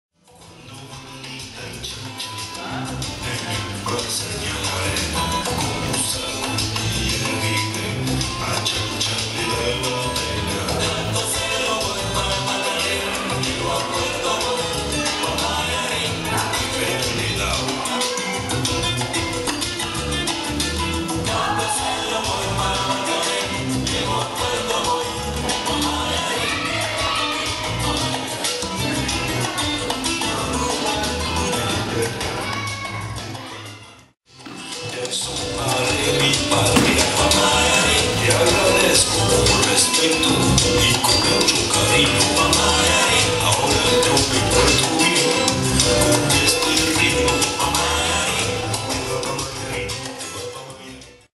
Sombrero Arrojar - Southern Cross Salsa
Video of the salsa dance figure "Sombrero Arrojar" for students